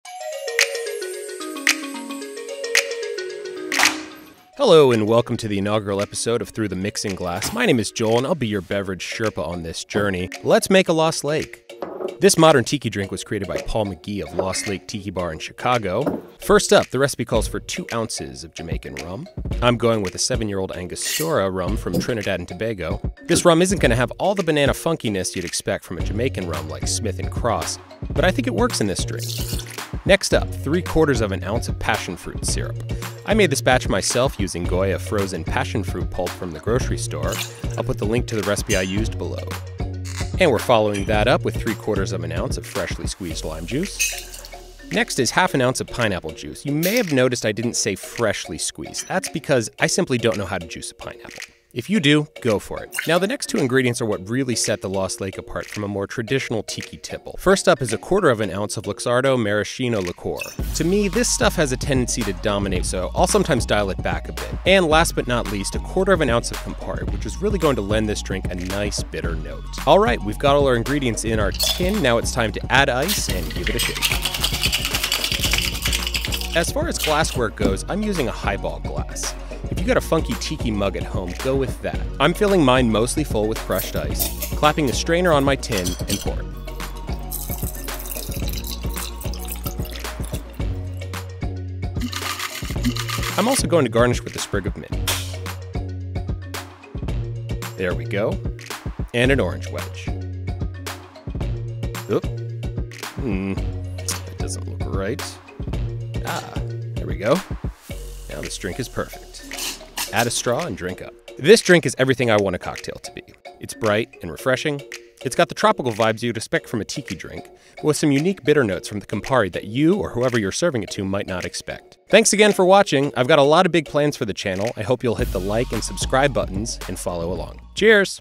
LOST LAKE: A MODERN TIKI MASTERPIECE - THROUGH THE MIXING GLASS
I love cocktails. You love cocktails. What I don't always love is spending close to $20 a drink at a fancy cocktail bar in the city.

So I'm building out my home bar and taking matters into my own hands. First up is Paul McGee's Lost Lake cocktail from the Lost Lake tiki bar in Chicago.

Ingredients
2 oz Jamaican rum
1/4 oz Luxardo maraschino liqueur
1/4 oz Campari
3/4 oz lime juice
3/4 oz passionfruit syrup
1/2 oz pineapple juice

- Add all ingredients to shaker tin and shake with ice
- Serve over crushed ice and garnish extravagantly!

Follow Paul on Instagram!